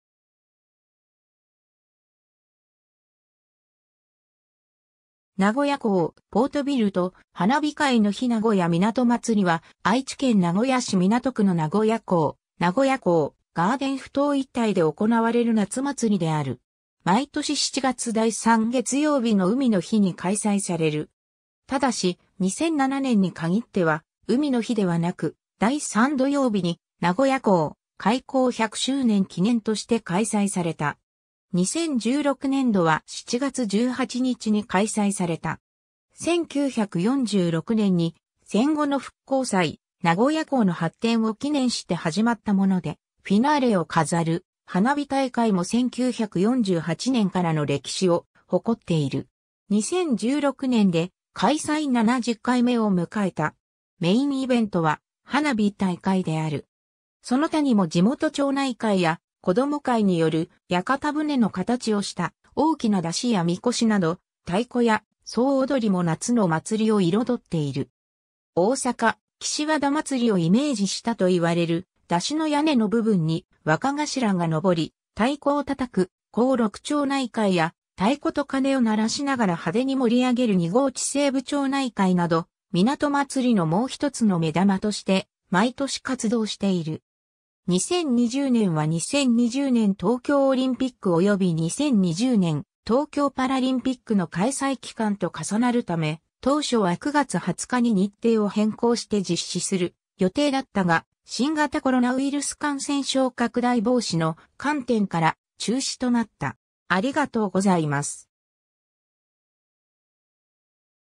海の日名古屋みなと祭
港区_(名古屋市)
名古屋港ポートビルと花火（2009年） 海の日名古屋みなと祭（うみのひなごやみなとまつり）は、愛知県名古屋市港区の名古屋港・名古屋港ガーデンふ頭一帯で行われる夏祭りである。
 毎年7月第3月曜日の海の日に開催される。
ただし、2007年に限っては海の日ではなく第3土曜日に、「名古屋港開港100周年記念」として開催された。
 2016年度は7月18日（月・祝）に開催された。
 1946年（昭和21年）に、戦後の復興祭・名古屋港の発展を祈念して始まったもので、フィナーレを飾る花火大会も1948年（昭和23年）からの歴史を誇っている。
2016年で、開催70回目を迎えた。
 メインイベントは花火大会（メモリアル花火など）である。
その他にも地元町内会や子供会（西築地学区）による屋形舟の形をした大きな山車や神輿など、太鼓や総踊りも夏の祭りを彩っている。
大阪・岸和田祭りをイメージしたと言われる、山車の屋根の部分に若頭が上り太鼓を叩く高六町内会や、太鼓と鐘を鳴らしながら派手に盛り上げる二号地西部町内会など、みなと祭のもうひとつの目玉として毎年活動している。
 2020年（令和2年）は2020年東京オリンピックおよび2020年東京パラリンピックの開催期間と重なるため、当初は9月20日に日程を変更して実施する予定だったが、新型コロナウイルス感染症拡大防止の観点から、中止となった（なお、東京オリンピック・パラリンピックも、2021年に開催延期が決まっている）。